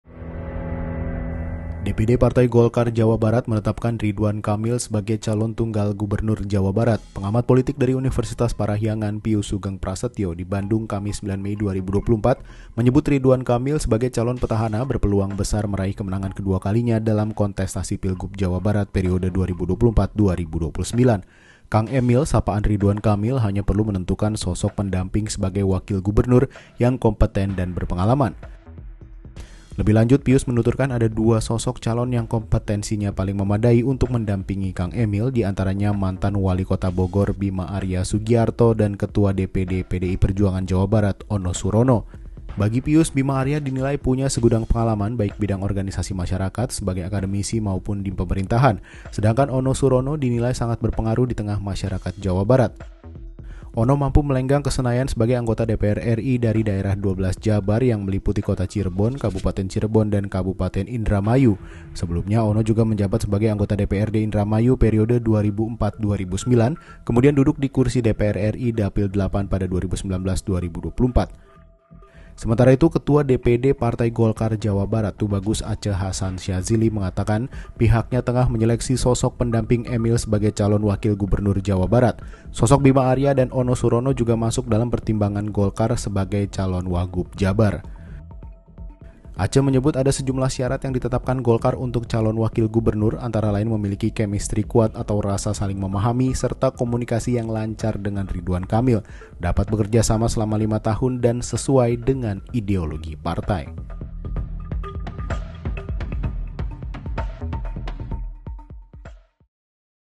Golkar Ajukan Ridwan Kamil di Pilgub Jabar, Ono Surono atau Bima Arya Bakal Dampingi
DPD Partai Golkar Jawa Barat menetapkan Ridwan Kamil sebagai calon tunggal gubernur Jawa Barat. Pengamat politik dari Universitas Parahyangan Pius Sugeng Prasetyo, di Bandung, Kamis (9/5/2024), menyebut Ridwan Kamil sebagai calon petahana berpeluang besar meraih kemenangan kedua kalinya dalam kontestasi Pilgub Jawa Barat periode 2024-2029. Kang Emil, sapaan Ridwan Kamil, hanya perlu menentukan sosok pendamping sebagai wakil gubernur yang kompeten dan berpengalaman.

Sahabat Kompas bisa baca artikel lainnya di tautan berikut:
Bima Arya dan Ono Surono, Calon Kuat Pendamping Ridwan Kamil di Jabar